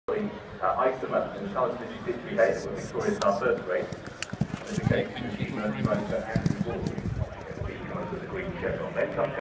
Frankel two year old Cunco at Newbury.
Frankel's first ever runner heads to post at Newbury 13 May 2016.